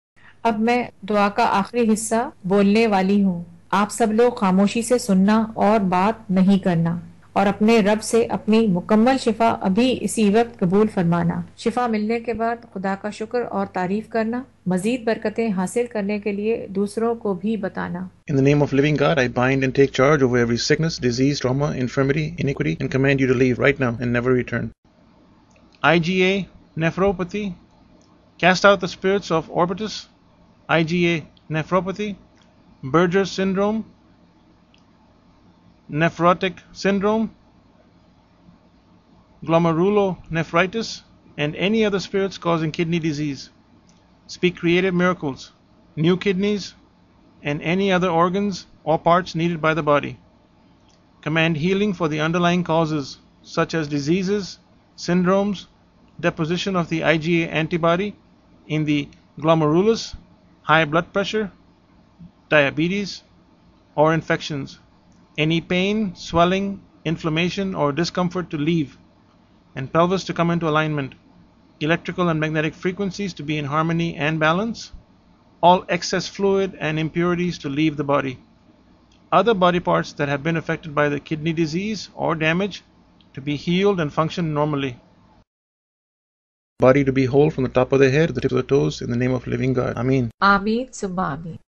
IgA Nephropathy - Inflammation of Glomeruli of Kidney
IgA nephropathy (also known as IgA nephritis, IgAN, Berger's disease, Berger's syndrome and synpharyngitic glomerulonephritis) is a form of glomerulonephritis (inflammation of the glomeruli of the kidney).